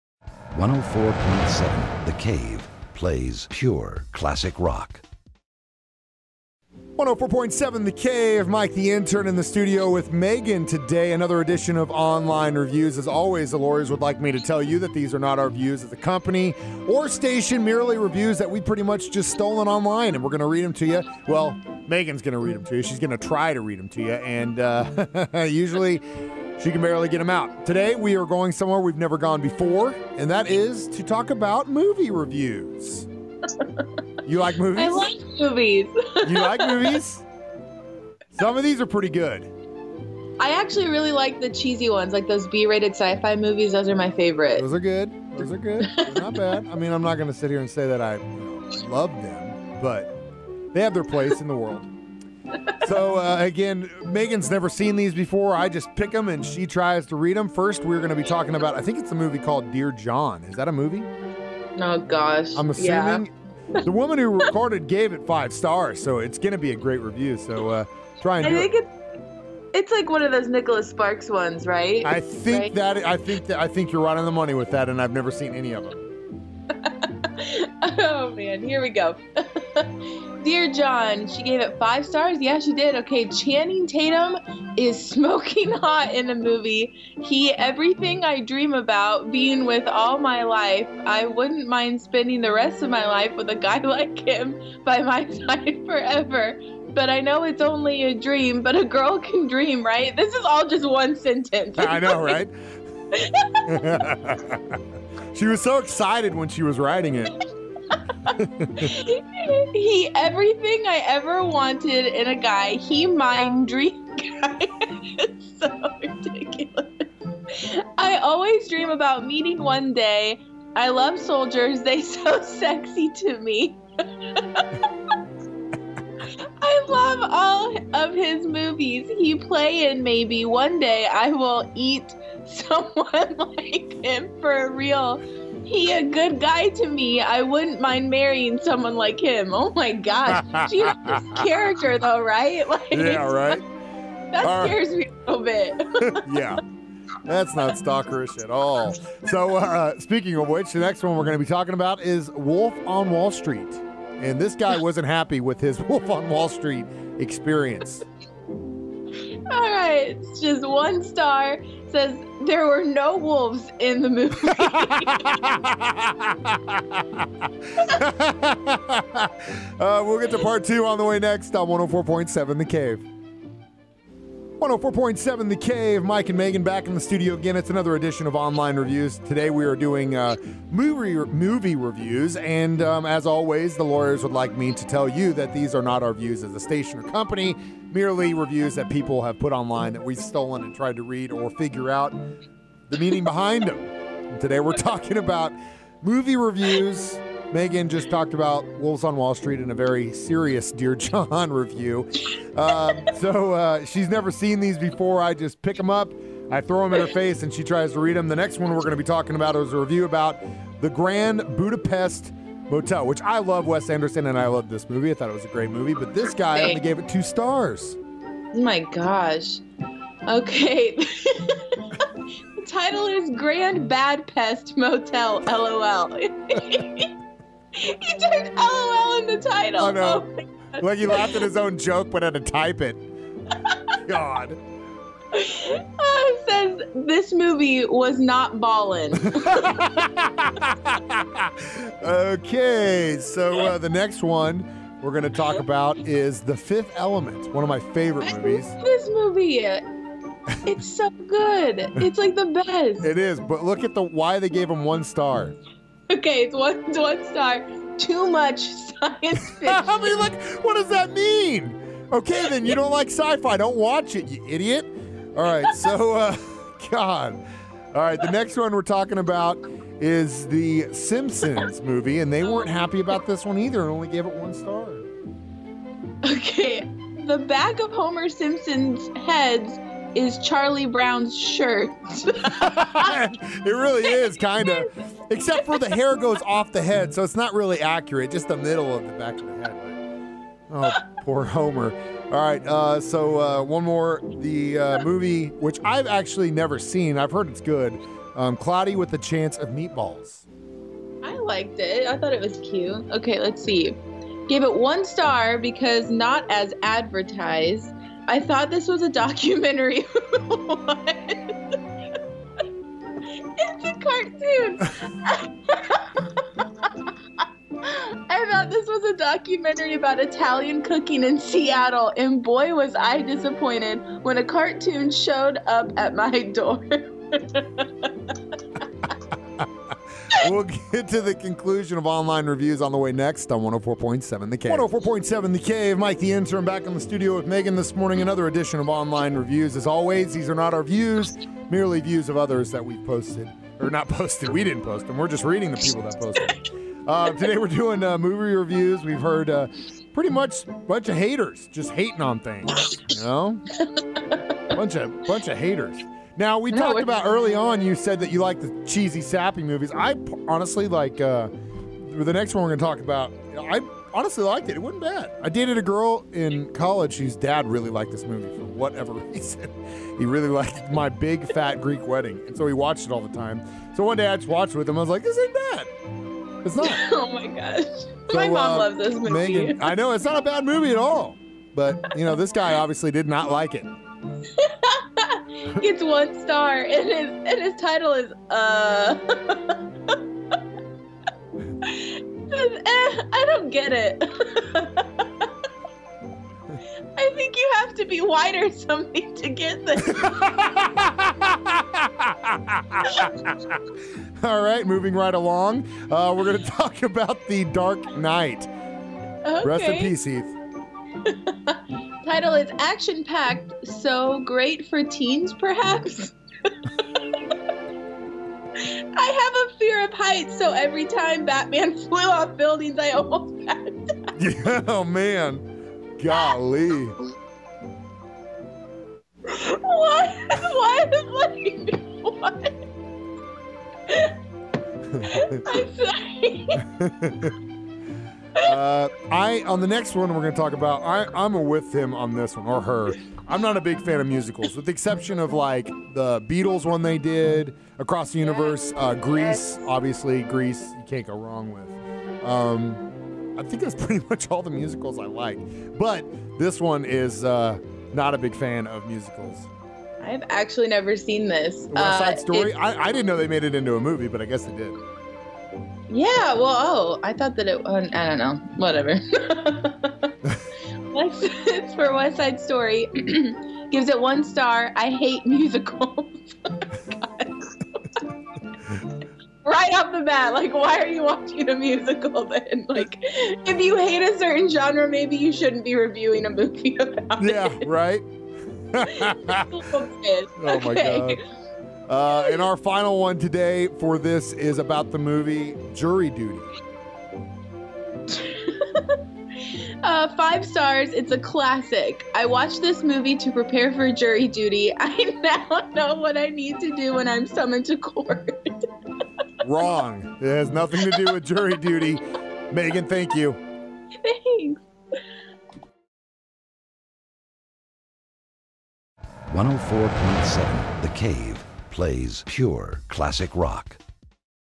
Online Reviews 4.27.17 Movie Reviews!
Mike and Meghan talk about Movie Reviews!